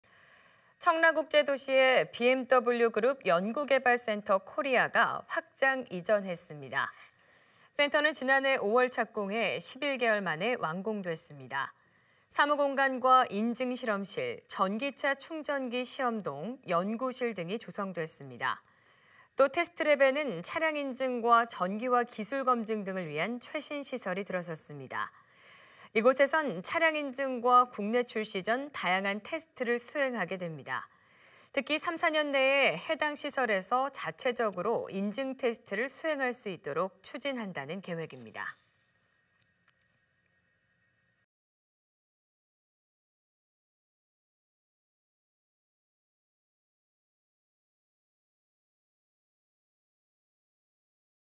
청라에 'BMW R&D 센터 코리아' 개관
[기사내용]
청라국제도시에 BMW 그룹 연구개발 센터 코리아가 확장 이
전했습니다.

센터는 지난해 5월 착공해 11개월 만에 완공됐습니다.

사무 공간과 인증 실험실, 전기차 충전기 시험동, 연구실
등이 조성됐습니다.

또 테스트랩에는 차량 인증과 전기화 기술 검증 등을 위한
최신 시설이 들어섰습니다.

이곳에선 차량 인증과 국내 출시 전 다양한 테스트를 수행하
게 됩니다.

특히 3~4년 내에 해당 시설에서 자체적으로 인증 테스트를
수행할 수 있도록 추진한다는 계획입니다.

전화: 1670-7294